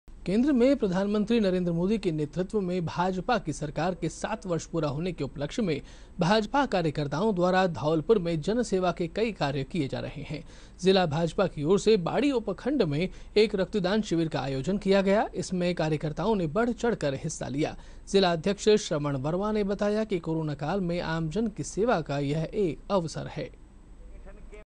भाजपा कार्यकर्ताओं ने रक्तदान शिविर का आयोजन किया
भाजपा कार्यकर्ताओं की ओर से बाड़ी में रक्तदान शिविर का आयोजन किया गया है। केन्द्र में भाजपा सरकार के सात वर्ष की अवधि पूर्ण होने पर यह आयोजन किया गया है।